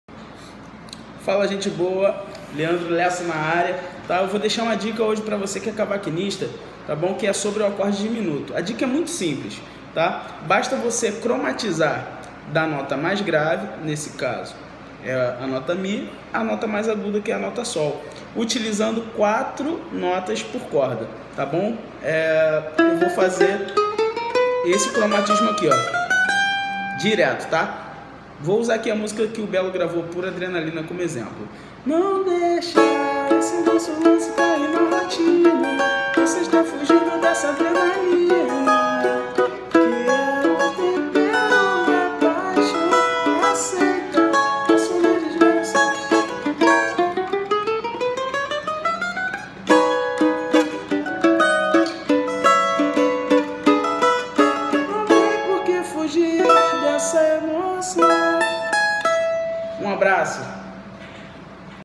Acordes Diminutos - dicas de improvisação 1
A dica da semana é sobre o acorde Diminuto; um acorde que tem inúmeras possibilidades de recursos para destacá-lo. Este vídeo abre um caminho para outros vídeos que serão postados posteriormente. Forte abraço e divirtam-se!!!